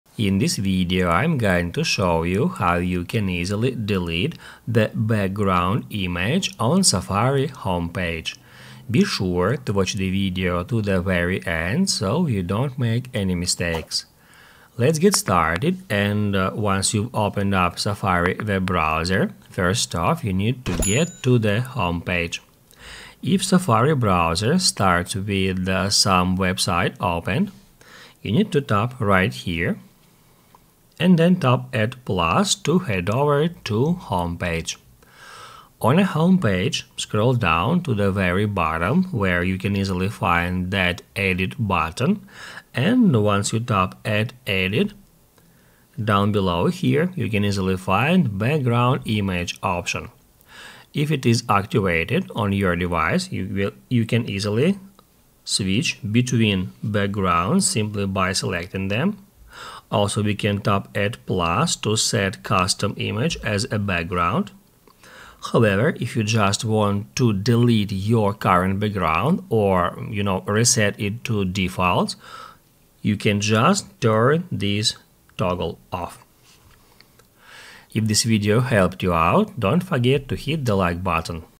How to Delete Background Image on Safari Home Page
If you are looking for a video about how to delete background image on safari home page, here it is! In this video I will show you how to change background in safari. Be sure to watch the video to the very end.